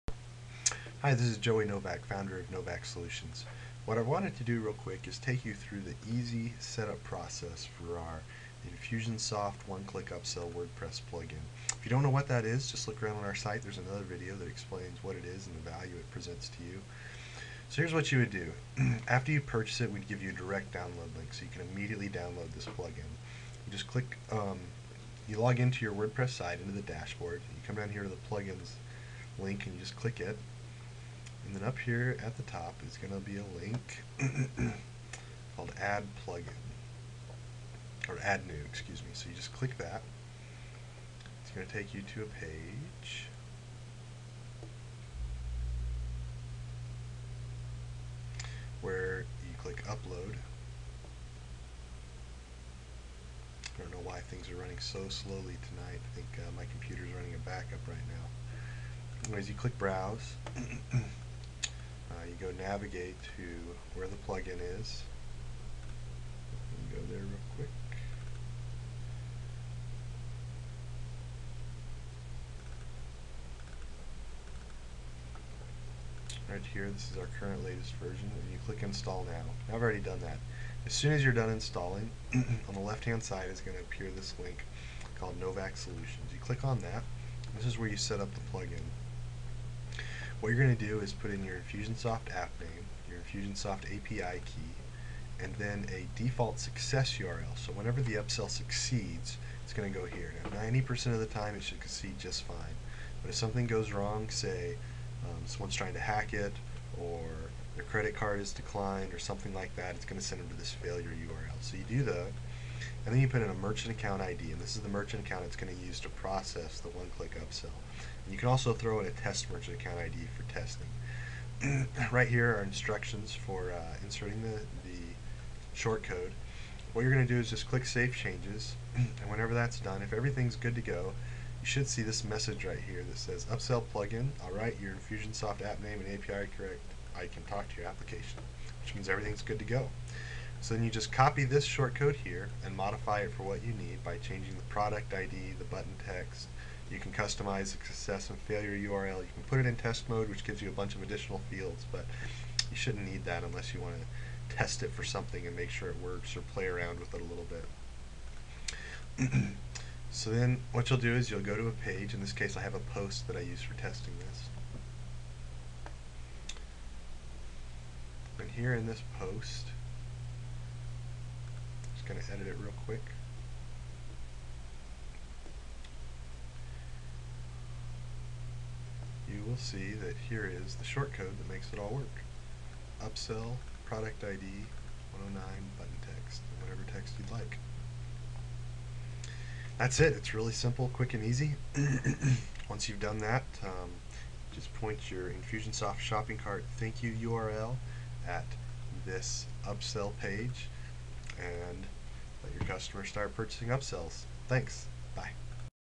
Infusionsoft(R) One Click Upsell Wordpress Plugin - Installation
Easily install the one click Upsell Wordpress Plugin to give your customers an EASY way to upsell when the complete an Infusionsoft Checkout.